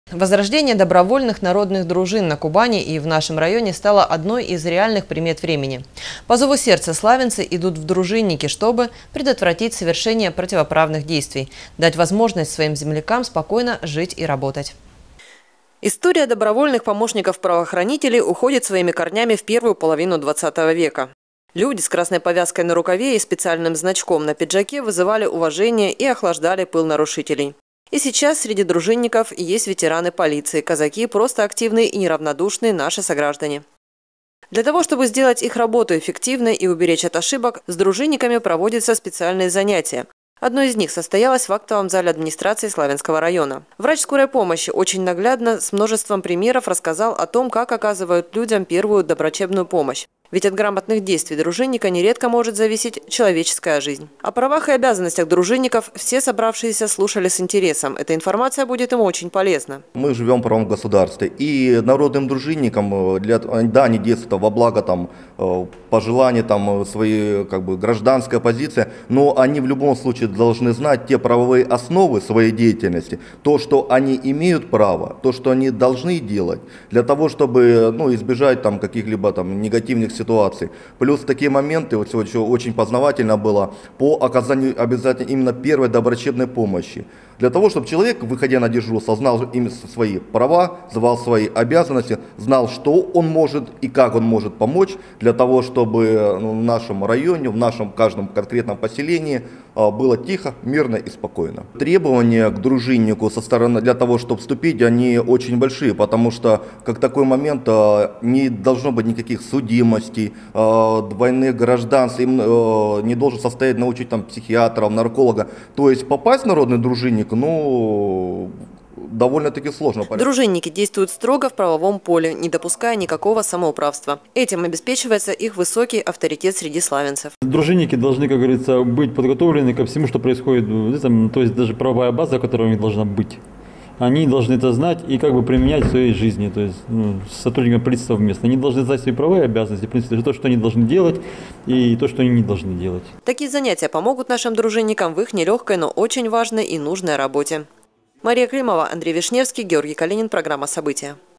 На страже порядка и спокойствия жителей
Возрождение добровольных народных дружин на Кубани и в нашем районе стало одной из реальных примет времени. По зову сердца славянцы идут в дружинники, чтобы предотвратить совершение противоправных действий, дать возможность своим землякам спокойно жить и работать.